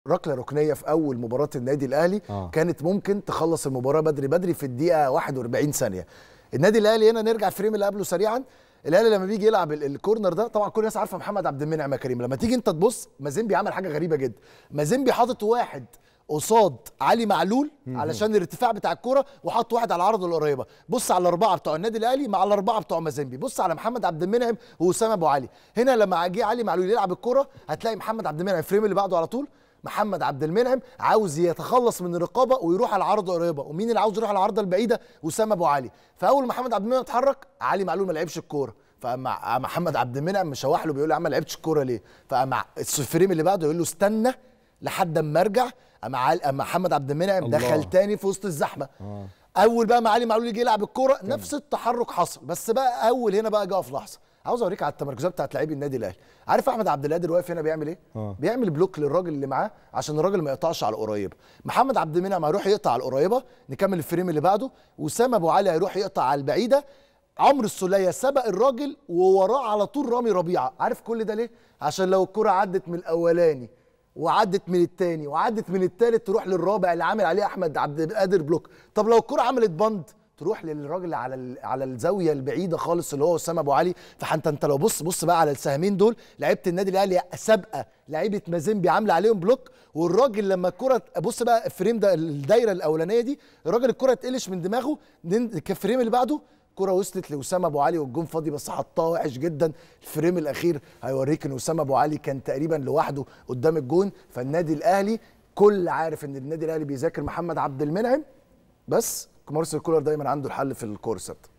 ركنية كانت هتخلص الماتش من بدري.. محمود شاكر هيوريك تكتيك هيفاجئك بالأهلي في الكرات الثابتة
💣💥 ركنية كانت هتخلص الماتش من الدقيقة التانية 👌.. محمود شاكر هيوريك تكتيك هيفاجئك 😲 من الأهلي في الركنيات والكرات الثابتة 💣💥🔥